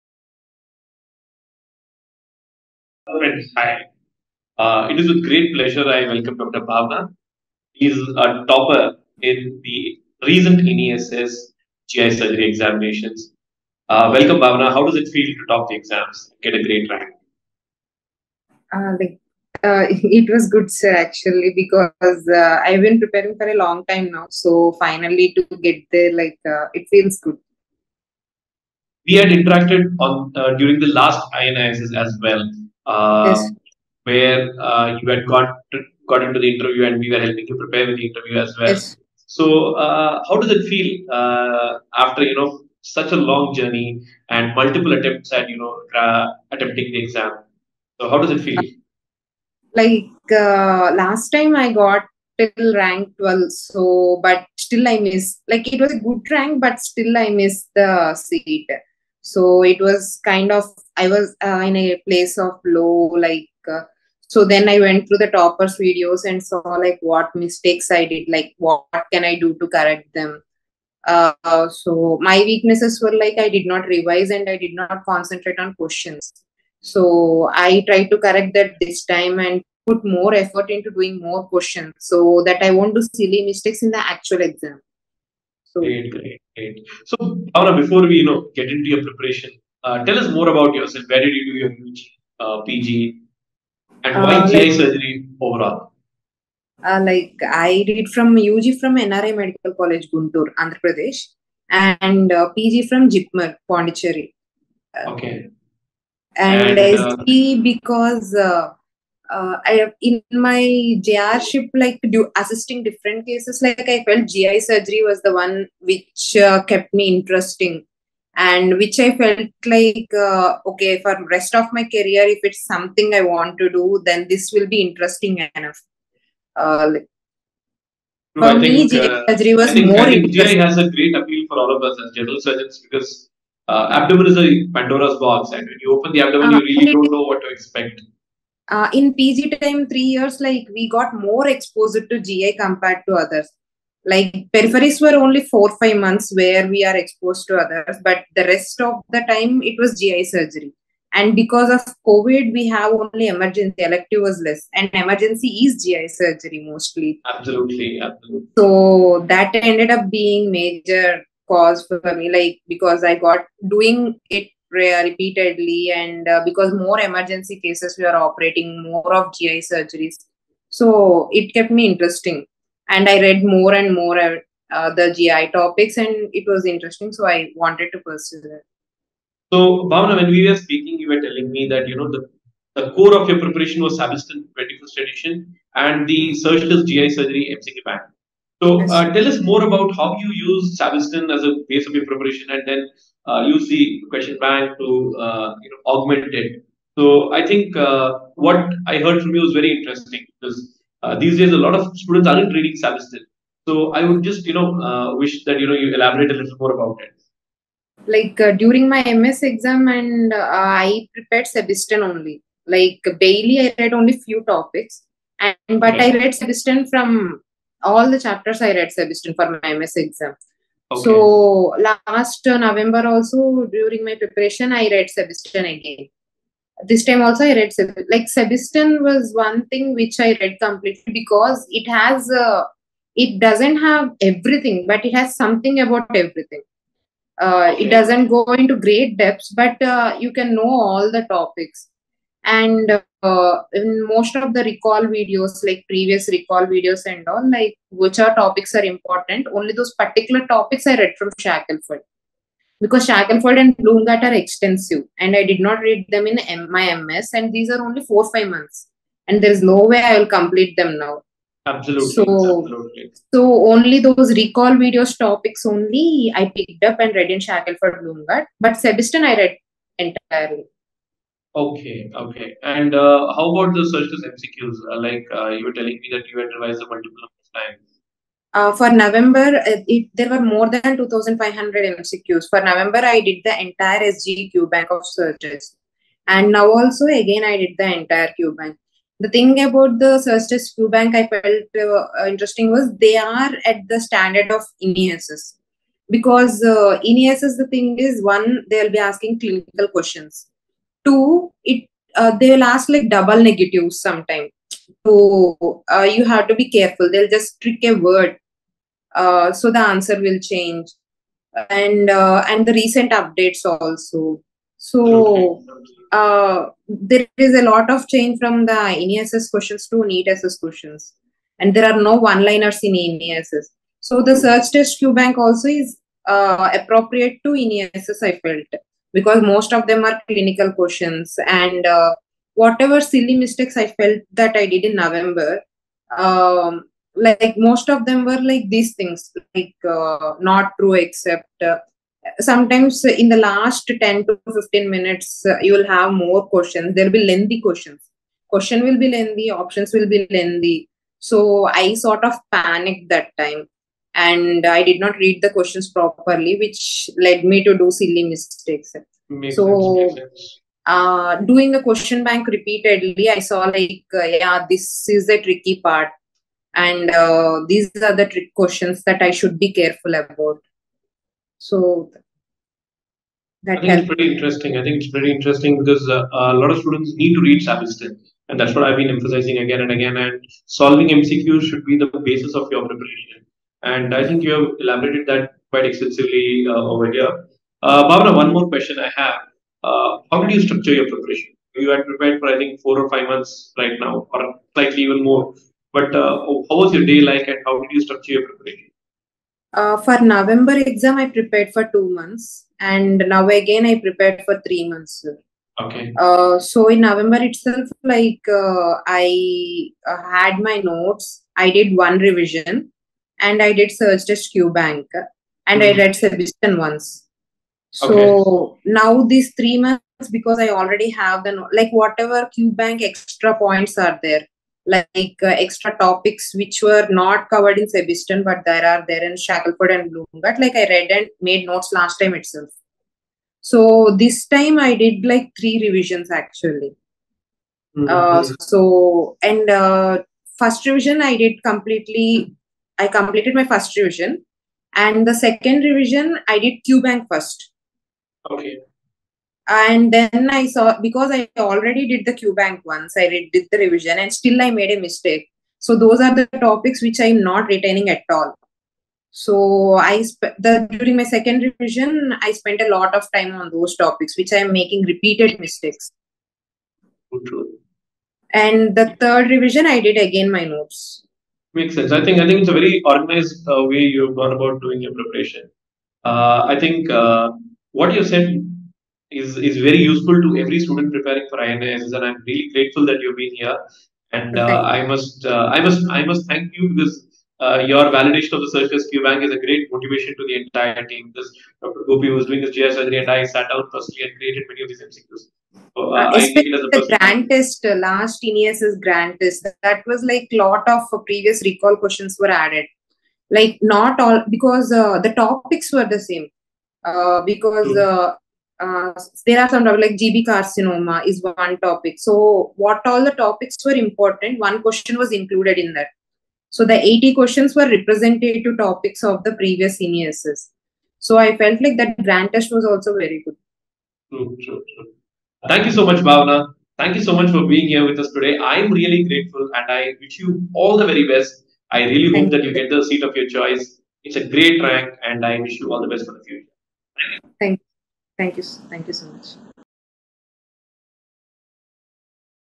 INI SS April 2023 Exams: AML Rank 4 in GI Surgery | Topper Interview with Dr. Katta Bhavana
Dr. Vinayak S Rengan and Dr. Katta Bhavana discuss how she secured Rank 4 in GI Surgery in INI SS April 2023. Watch this interview to know about her preparation strategies & her interest in GI Surgery.

0:00 - 1:29 - Introduction
1:30 - 2:58 - Journey and Background
2:58 - 4:30 - Core of Preparation & Resources Used
4:34 - 9:00 - Surgtest role in Preparation

Want to solve MCQs and watch videos, Download the Surgtest App